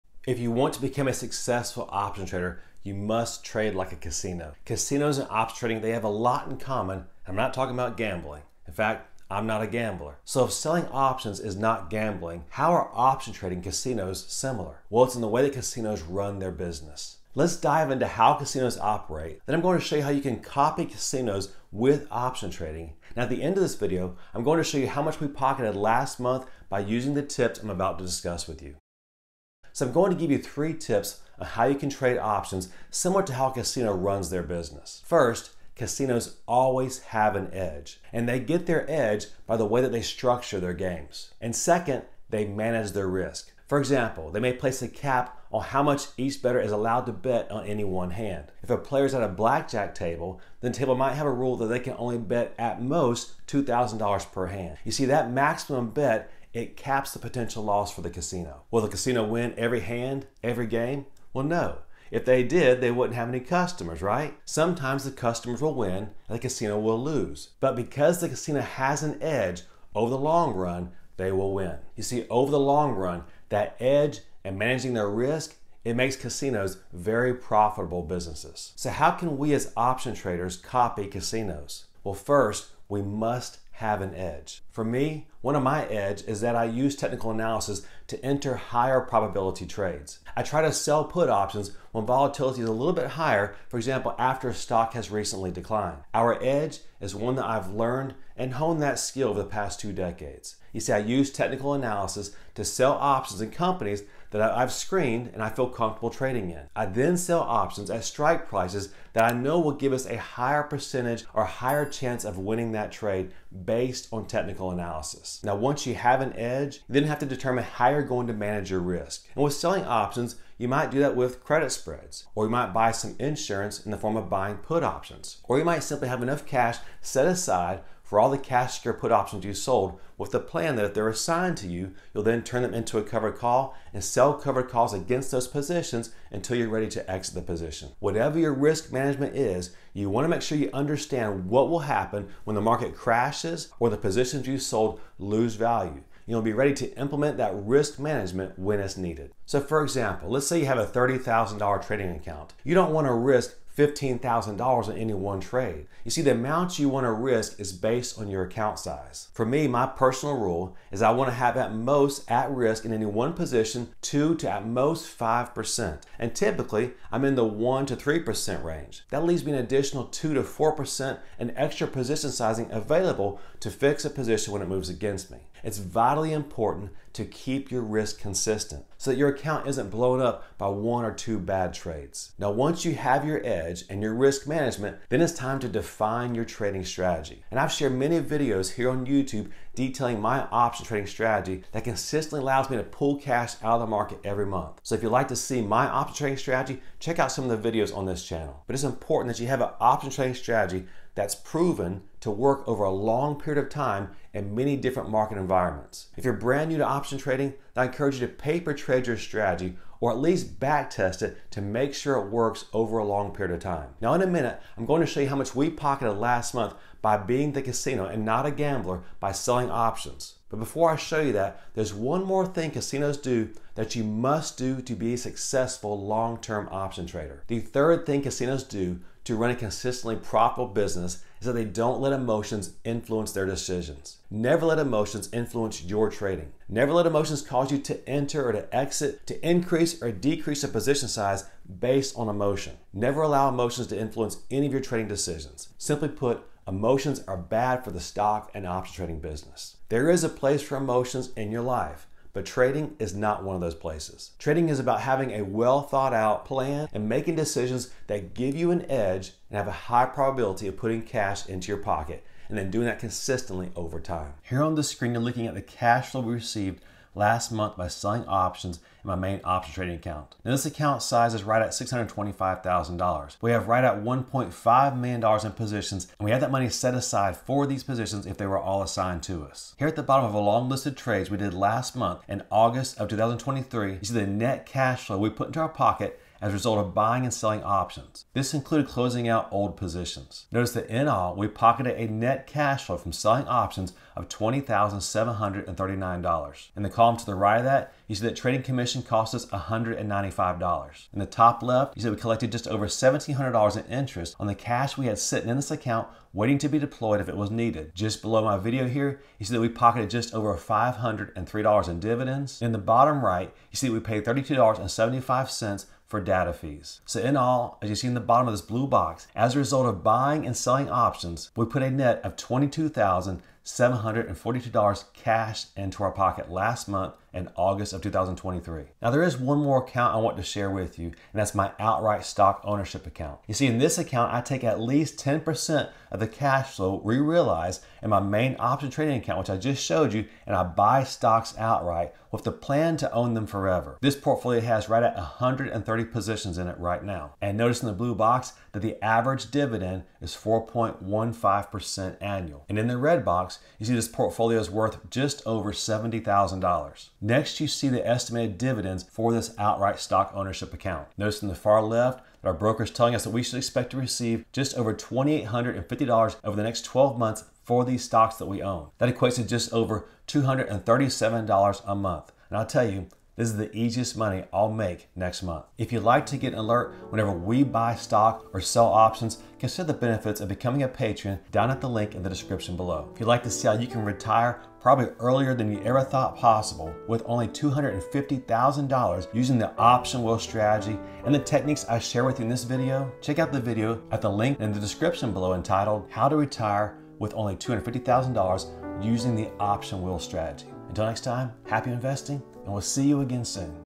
Option Trade Like A Casino to Make Money (August 2023 Cash Flow)
💰 BLACK FRIDAY SALE until midnight on Tuesday, 12/3/2024 eastern!

Chapters
0:00 Intro
0:35 How a Casino operates (3 keys)
1:18 How to Option Trade like a Casino?
1:40 (1) My Option Trading Edge
1:55 (2) Risk Management
2:39 How I Manage Risk
3:40 (3) Manage Emotions (Casino's do it and so should you)
4:24 Option Trading Cash Flow from August of 2023
5:28 Outright Dividend Stock Portfolio Cash Flow

To become a successful option trader, it's crucial to approach it like a casino. Let's explore how casinos operate and how you can apply similar principles to option trading:

**1. Find Your Edge:**
Casinos have an edge, which comes from how they structure their games. In option trading, you can use technical analysis to enter higher probability trades. Look for opportunities when volatility is higher than normal, such as after a recent stock decline. Your edge should be something you've learned and refined over time.

**2. Manage Risk:**
Casinos cap how much can be bet on any one hand to manage risk; for instance, in blackjack, there might be a rule limiting the maximum bet to $2,000 per hand. Similarly, in option trading, you must manage your risk. Consider using credit spreads, buying insurance with put options, or keeping cash aside to cover positions when selling put options.

**3. Emotions Have No Place:**
Casinos don't let emotions influence their decisions, and neither should you.

Now, let's see how much we pocketed last month using these strategies, staying true to the casino-like approach, where we're not gamblers but strategic option traders.

Happy investing!
Randy Perez